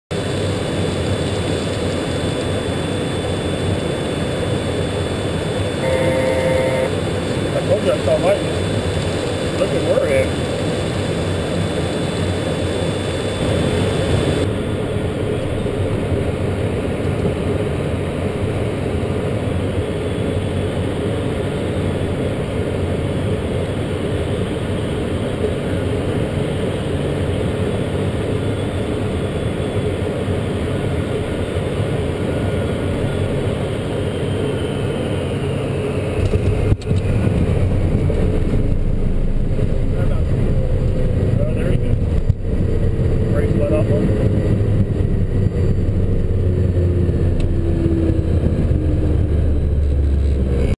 In Cloud Lightning
Found some lightning in the descent. Second clip is a landing with 30+ winds gusting to 45kts. Had my hands full.